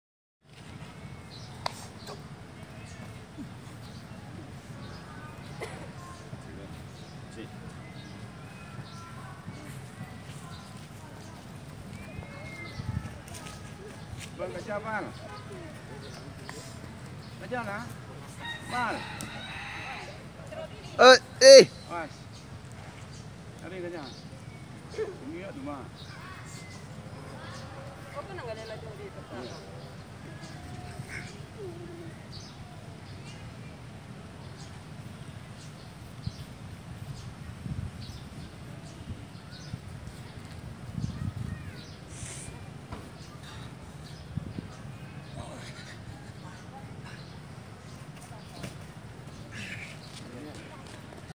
Ummi Upik (Parak Buluah) Dalam Latihan Rutin Budi Suci Kota Padang
Minggu Pagi, 22/07/18 di GOR H. Agus Salim - Kota Padang